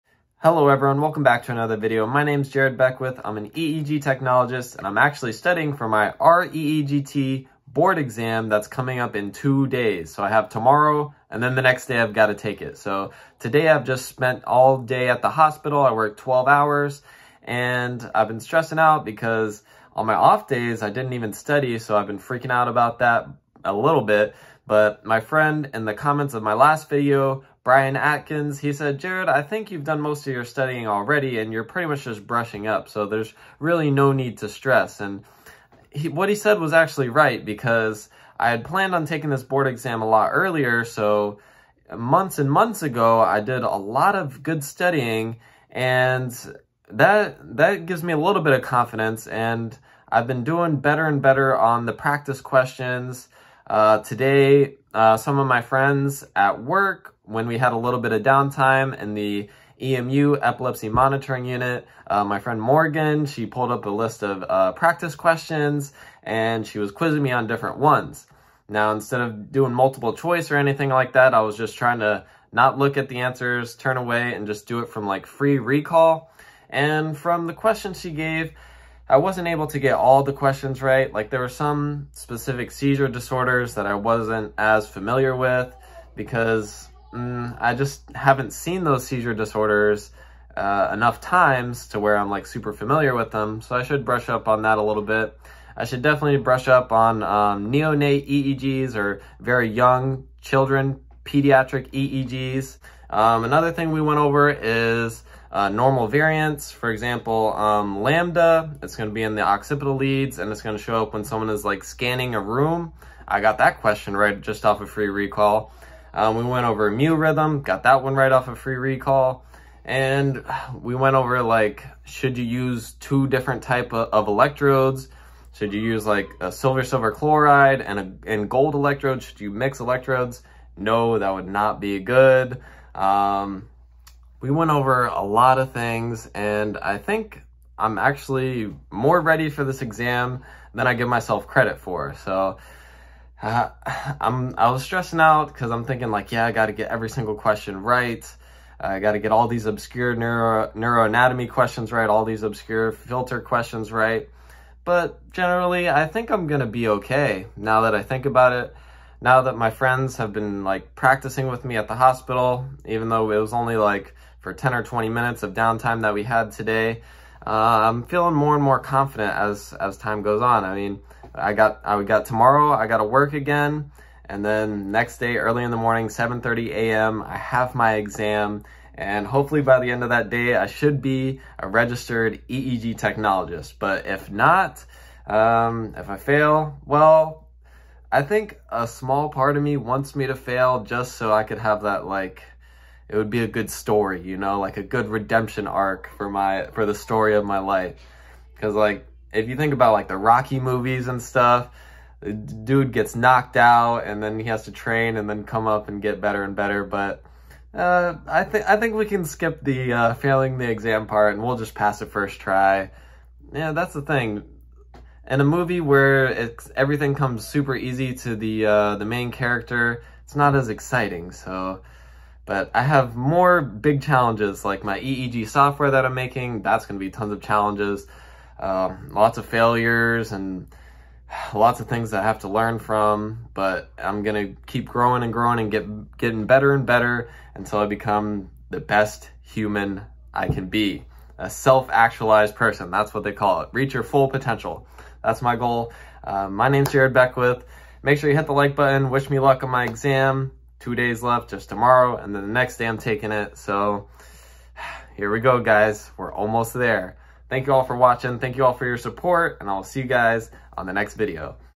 studying for the R. EEG T. exam day 2
My name is Jared Beckwith, I am an EEG technologist. In a few days I am taking the ABRET board exam to become a registered EEG technologist (R. EEG T.) here is my second day experience studying with my coworkers in our epilepsy monitoring unit.